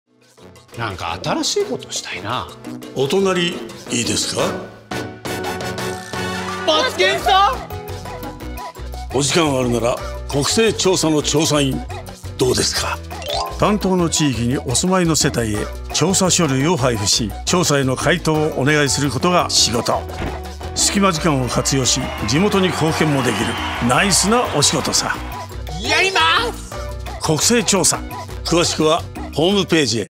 国勢調査 調査員募集動画「さぁ、ご一緒に！国勢調査員大募集」篇（30秒）
国勢調査は、国内の人及び世帯の実態を把握し、各種行政施策その他の基礎資料を得ることを目的とした、５年に一度の最も重要な統計調査です。
調査は、日本に住むすべての人と世帯（外国人の方も含む）を対象としています。

令和７年国勢調査は令和７年10月１日を調査期日として実施いたします。
市区町村では、「国勢調査員」として、調査業務に理解と誠意を持って携わっていただける方を広く募集しています。

詳しくは「国勢調査2025キャンペーンサイト」へ